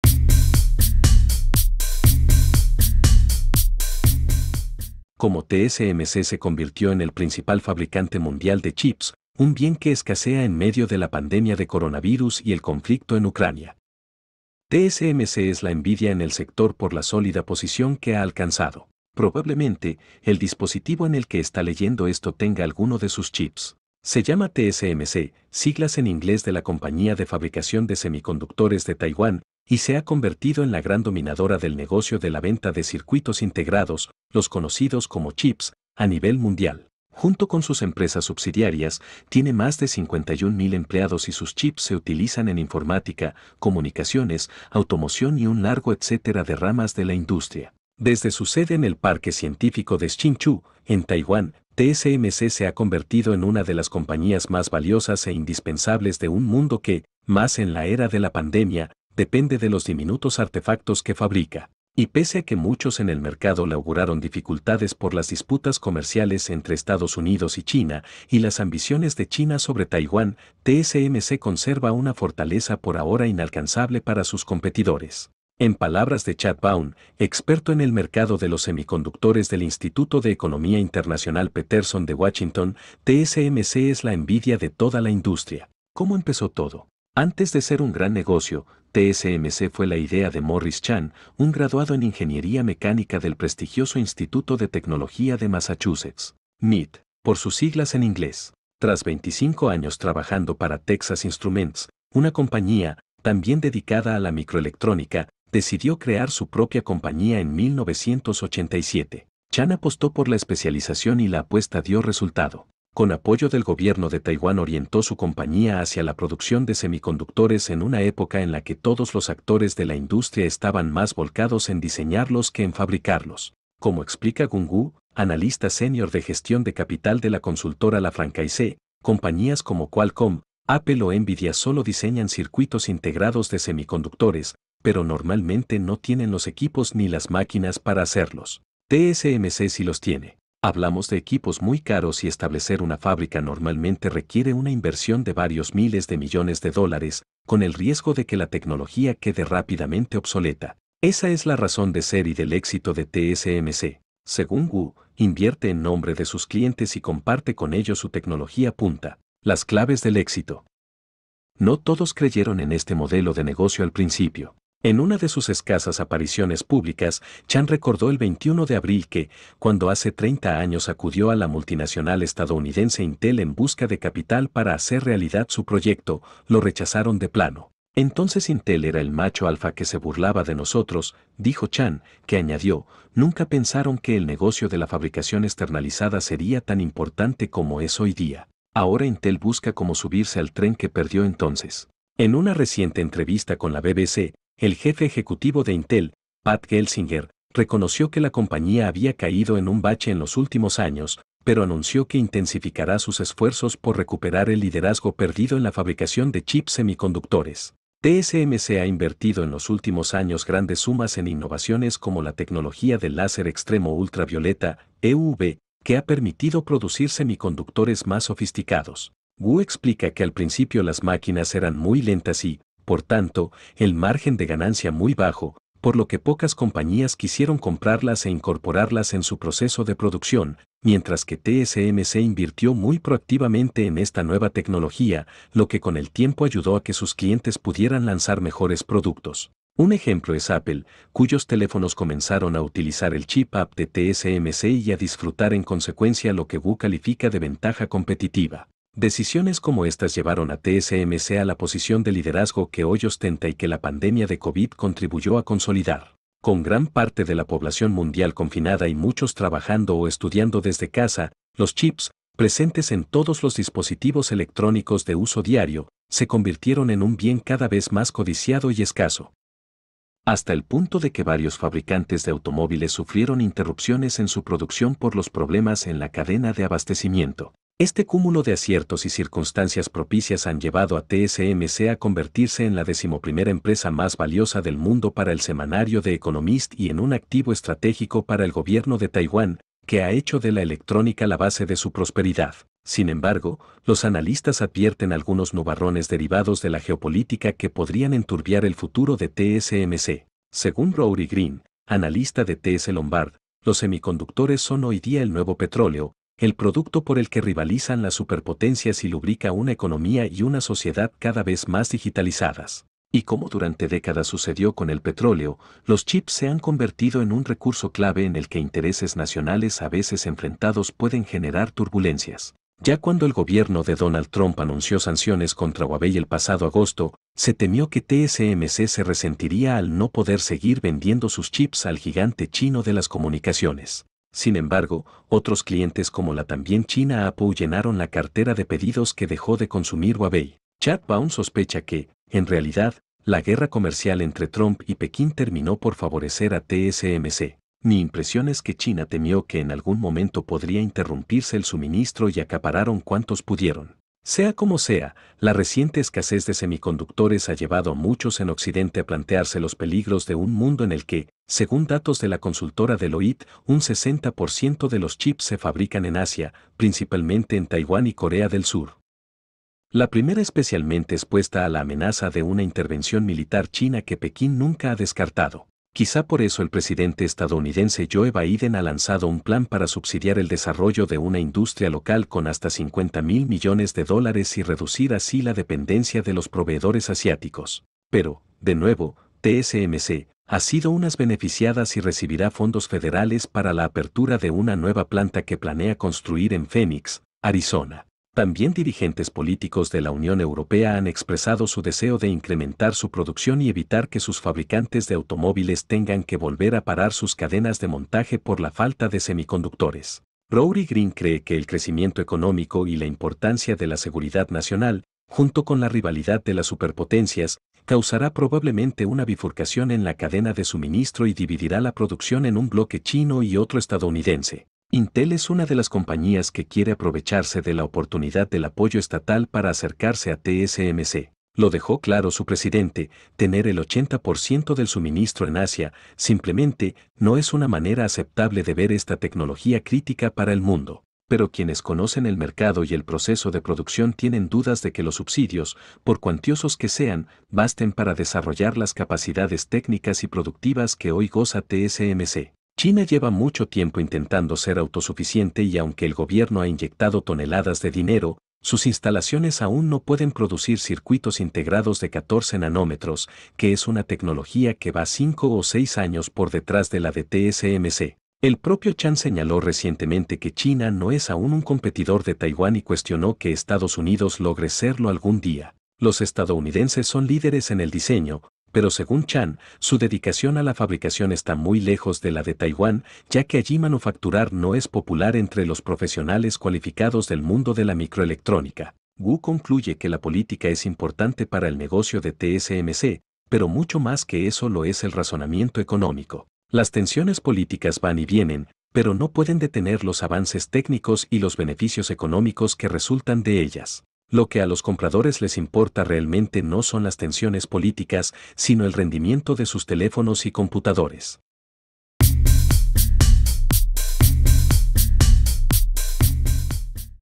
Cómo TSMC se convirtió en el principal fabricante mundial de chips?
Se llama TSMC (siglas en inglés de la Compañía de Fabricación de Semiconductores de Taiwán), y se ha convertido en la gran dominadora del negocio de la venta de circuitos integrados, los conocidos como chips, a nivel mundial.

Junto con sus empresas subsidiarias, tiene más de cincuenta y un mil empleados y sus chips se utilizan en informática, comunicaciones, automoción y un largo etcétera de ramas de la industria.

Desde su sede en el Parque Científico de Hsinchu, en Taiwán, TSMC se ha convertido en una de las compañías más valiosas e indispensables de un mundo que, más en la era de la pandemia, depende de los diminutos artefactos que fabrica.

Y pese a que muchos en el mercado le auguraron dificultades por las disputas comerciales entre Estados Unidos y China, y las ambiciones de China sobre Taiwán, TSMC conserva una fortaleza por ahora inalcanzable para sus competidores.